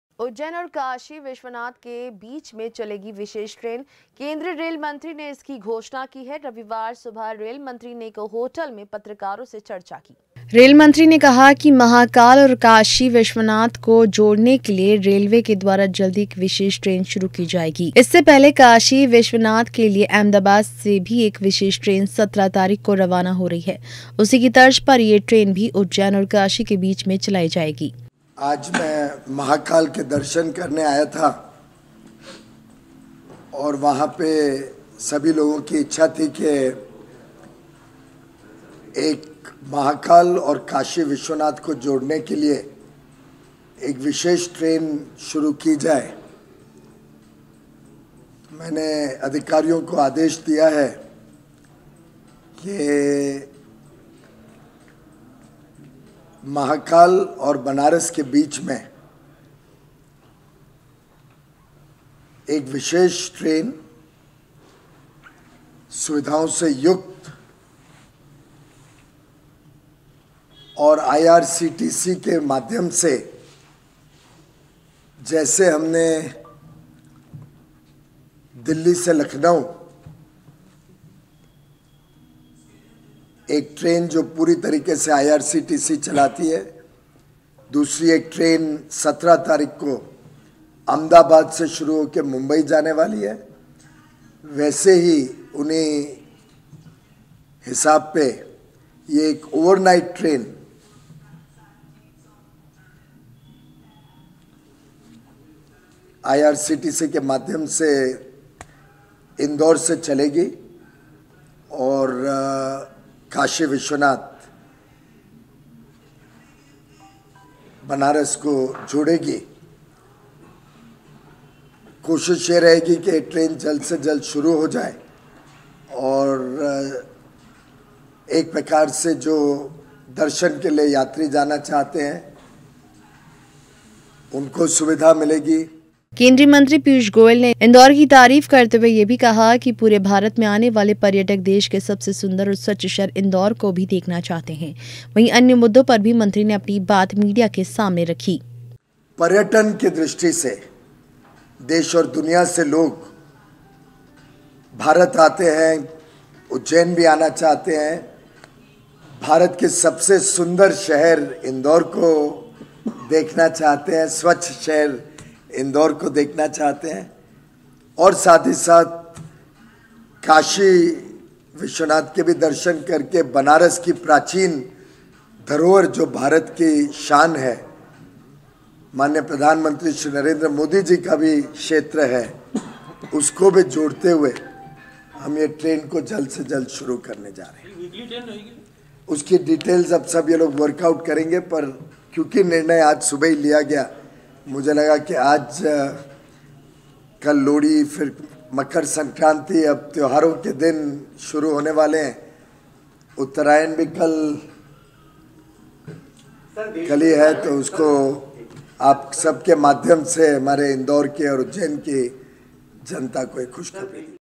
INDORE रेल मंत्री प्रेस कांफ्रेंस
उज्जैन और काशी विश्वनाथ के बीच में चलेगी विशेष ट्रेन। केंद्रीय रेल मंत्री ने इसकी घोषणा की है। रविवार सुबह रेल मंत्री ने एक होटल में पत्रकारों से चर्चा की।